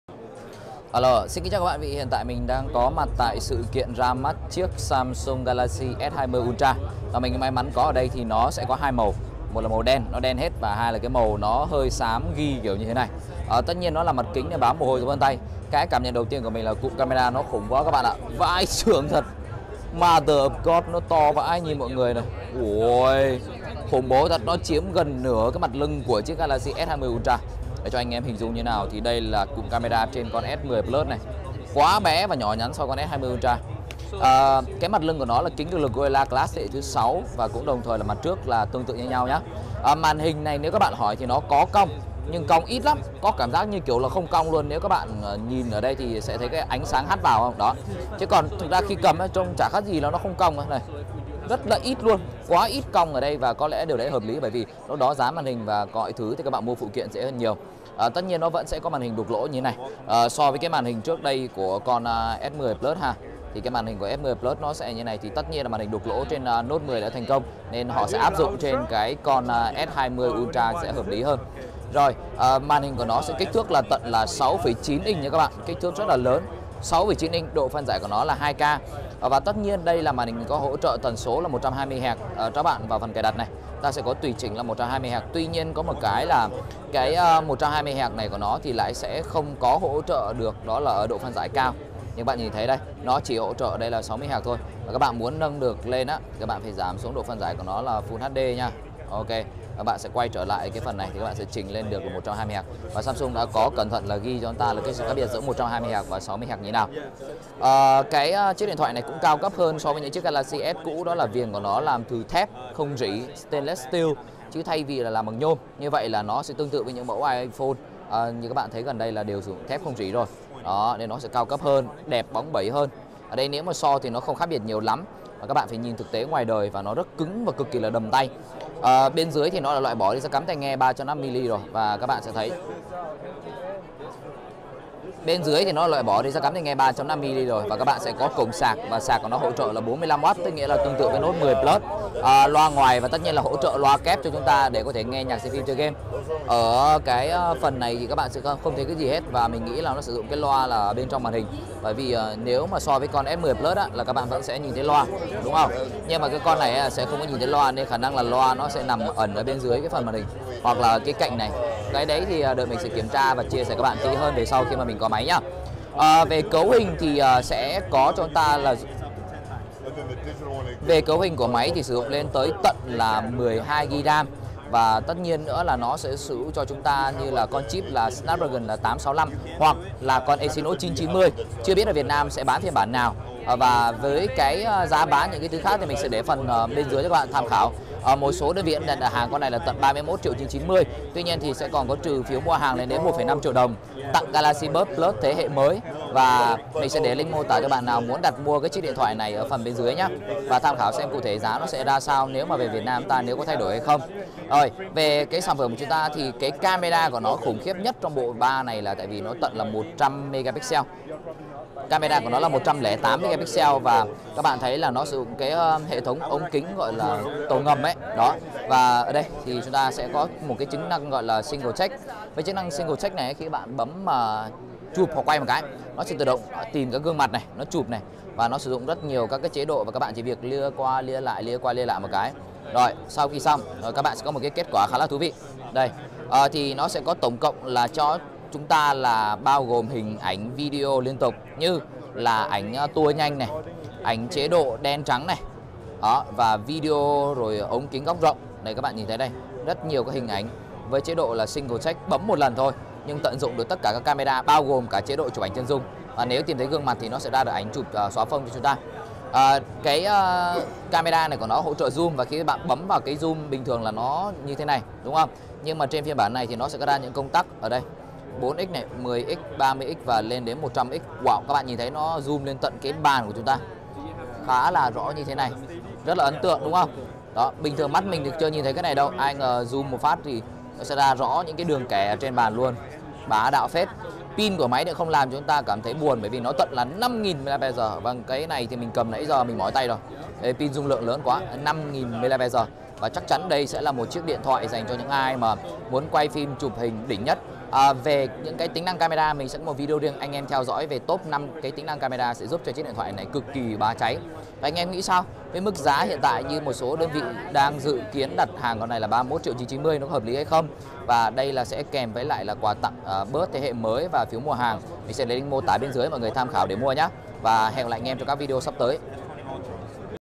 Đánh giá nhanh GALAXY S20 ULTRA 108MP, 5000mAh
Samsung Galaxy S20 Ultra chính là sản phẩm nổi bật nhất trong series Galaxy S20. Máy có mặt trước sau đều là kính cường lực gorilla glass 6, phần cạnh viền cũng cao cấp hơn được làm từ thép không rỉ. Sạc sẽ hỗ trợ lên đến 45W và đã loại bỏ jack 3,5mm. Cạnh viền cũng đã ít cong hơn so với S10, tất nhiên cấu hình cũng sẽ có hai phiên bản là Snapdragon 865 hoặc Exynos 990. Camera chính trên Samsung Galaxy S20 Ultra là điểm nhấn với cảm biến 108MP với rất nhiều tính năng thú vị. Galaxy S20 ultra cũng sẽ có phần viền thép chắc chắn so với viền nhôm như năm ngoái.

► Mua phụ kiện tai nghe, cáp sạc cho iPhone chính hãng

✌️FOLLOW TEAM VẬT VỜ STUDIO✌️